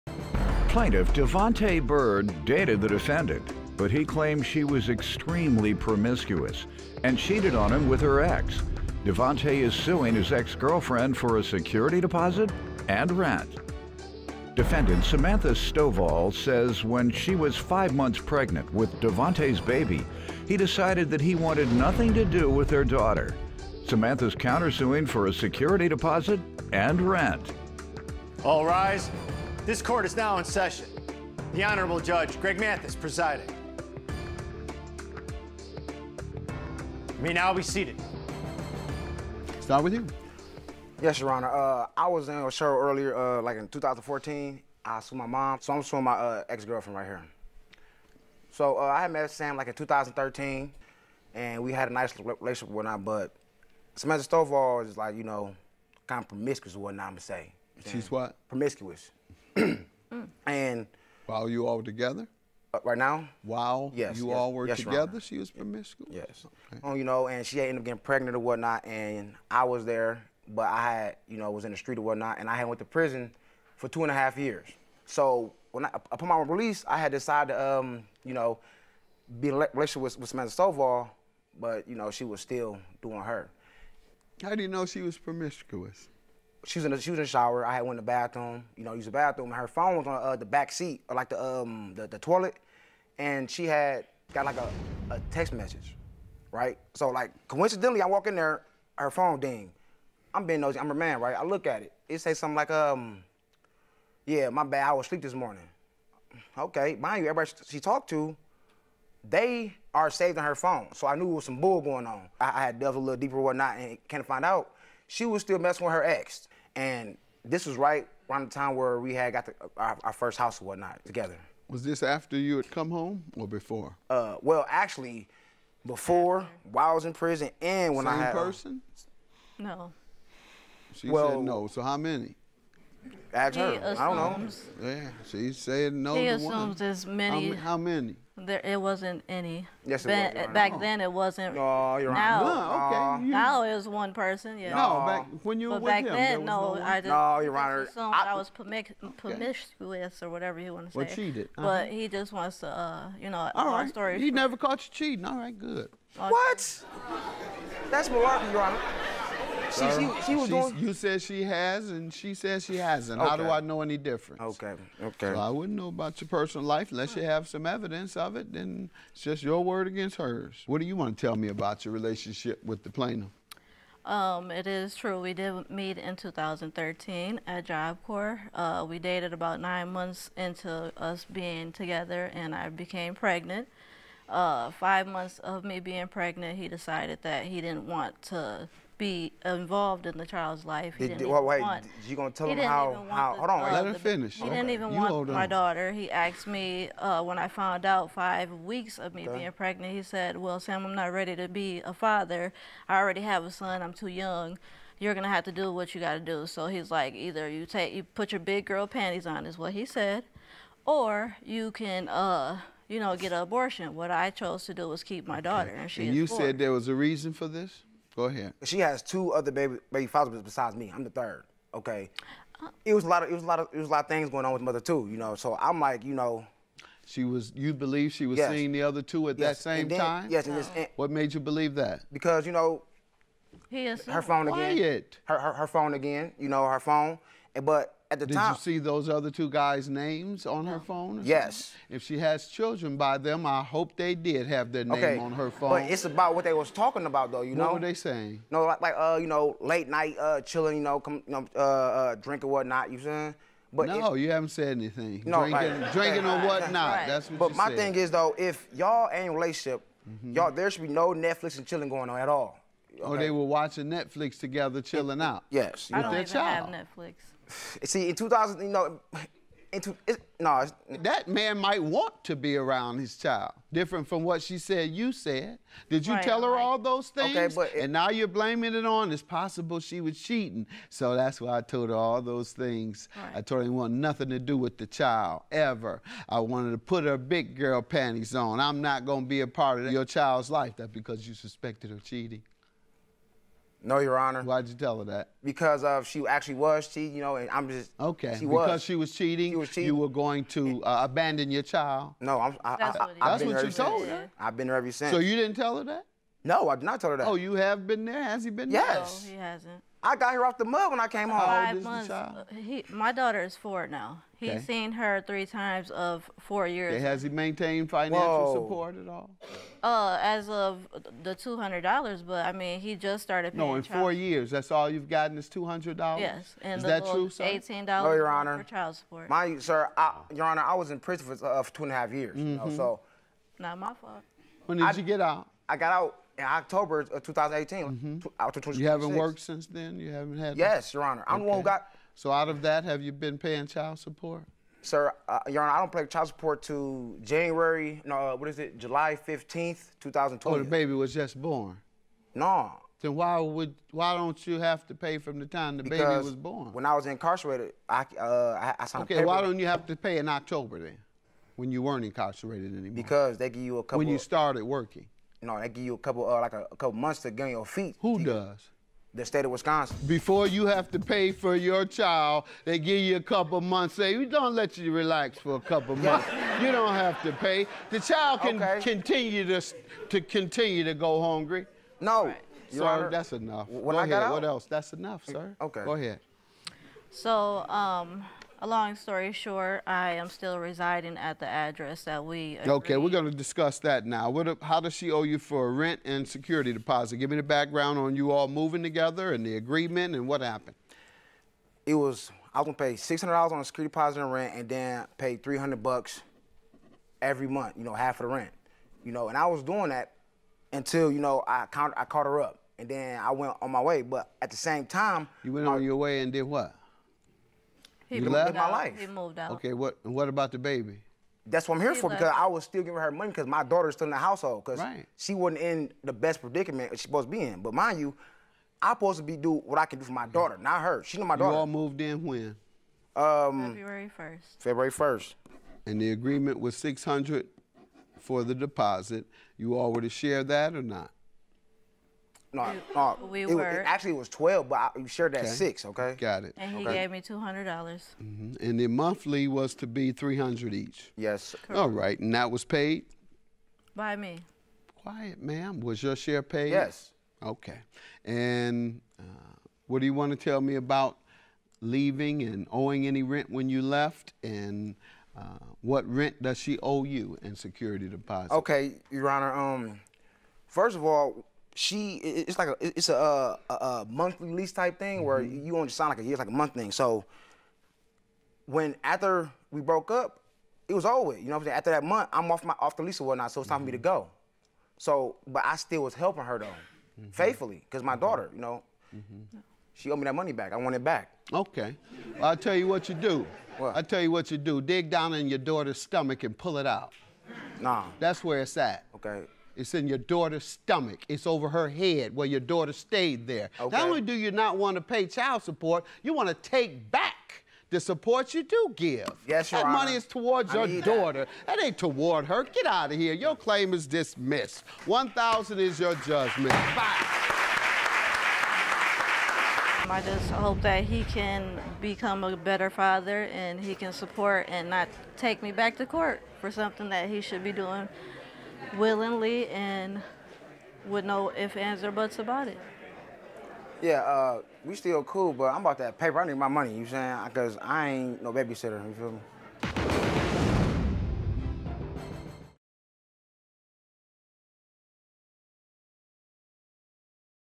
Why Don’t You Have to Pay Child Support?
The plaintiff claims his ex-girlfriend cheated on him with her ex, and he’s suing her for a security deposit and rent. The defendant claims the plaintiff abandoned her when she was five months pregnant. She’s countersuing for a security deposit and rent.